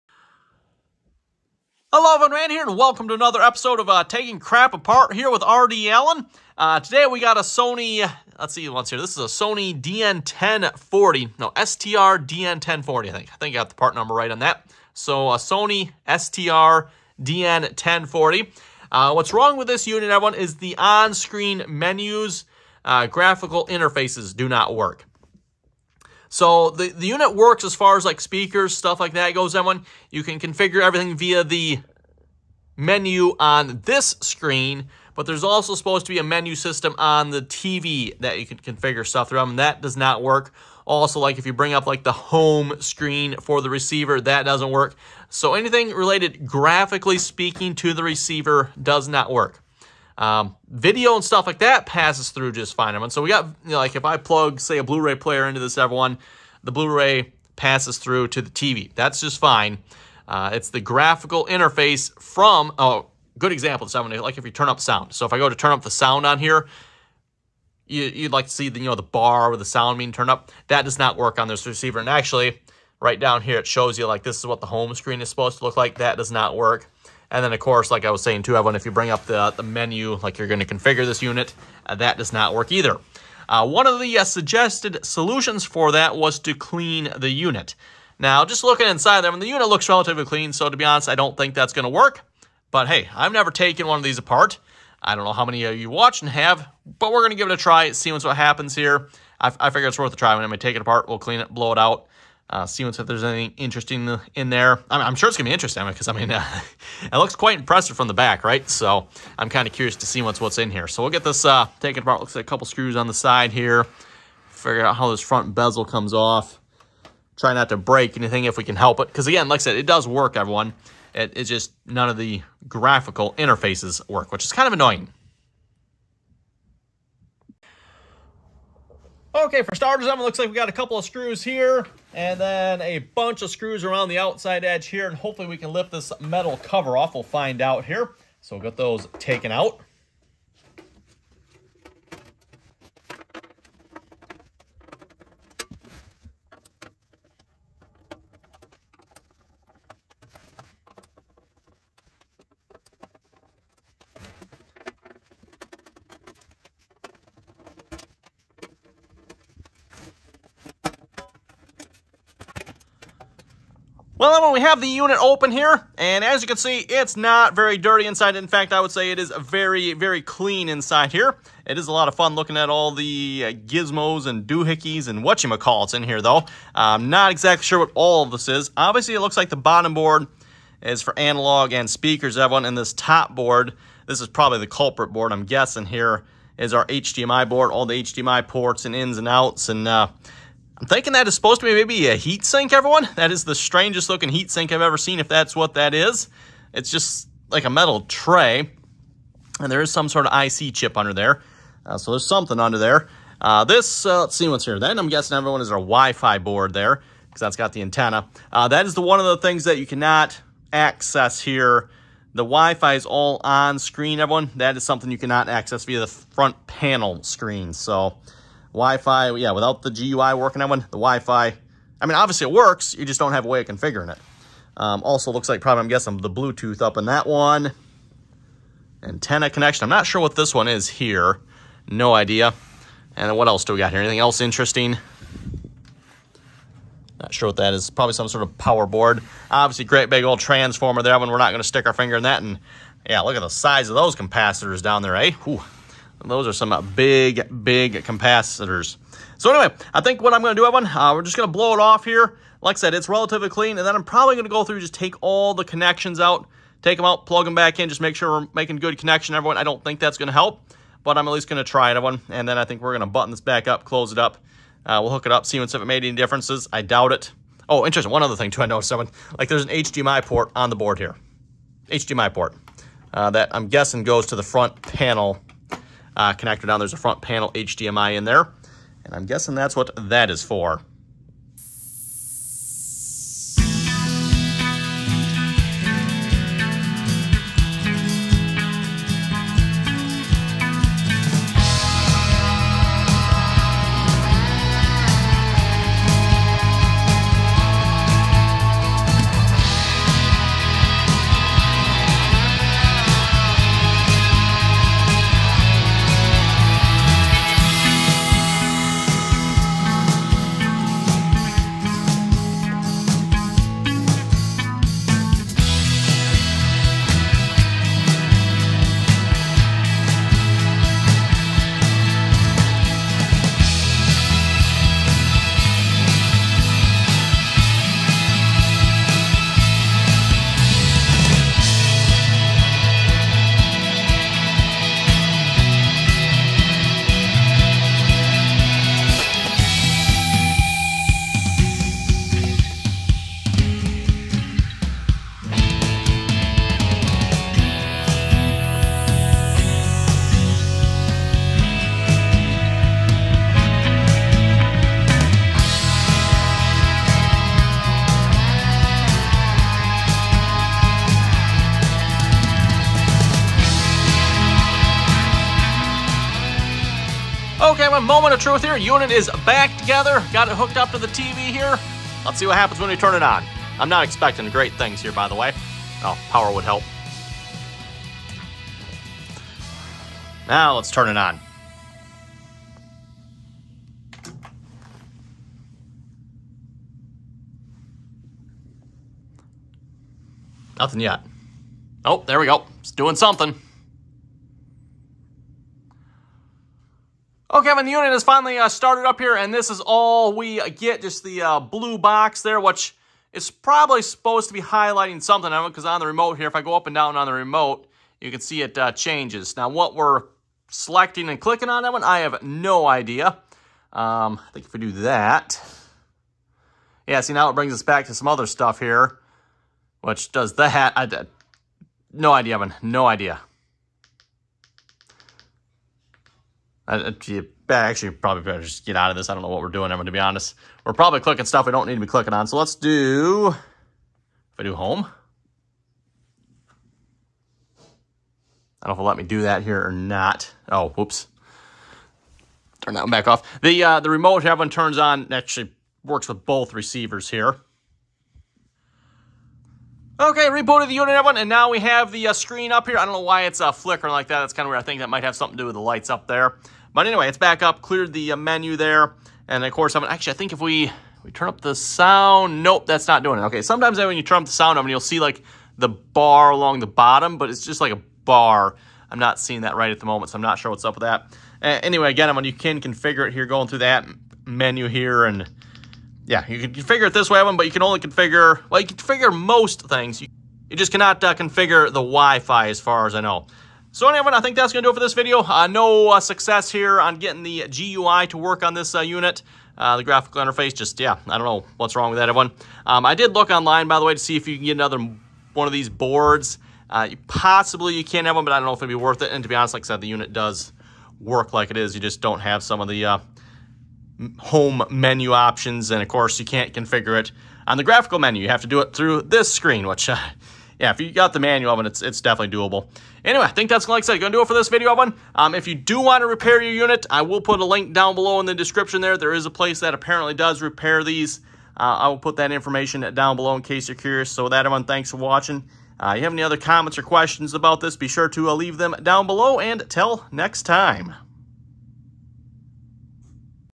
Attempting to Repair a Sony STR-DN1040 Receiver with No GUI
Looking to have your receiver repaired?
I have a Sony STR-DN1040 receiver that the receivers graphics do no not work on, one of the suggestions for fixing the problem is cleaning the unit. And when I say graphics don't work, I'm talking about anything on the receiver that generates on screen graphics, menu systems, volume level, home menu etc. Receiver passes video through just fine.